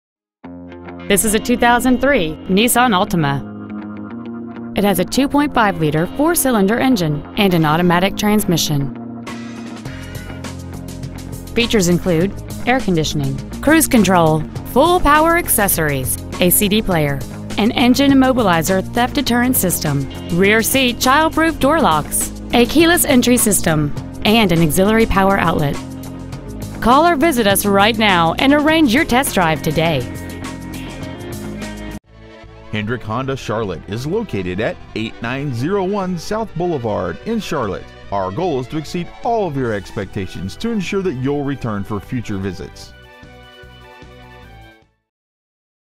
Used 2003 Altima Charlotte NC 28273
Year : 2003
 Make :
 Model : Altima
 Engine : 2.5L 4 cyl.
 Trans . : Automatic
 Exterior : Sonoma Sunset Pearl
 Miles : 110,519
 Interior : Charcoal
 Stock : 9926A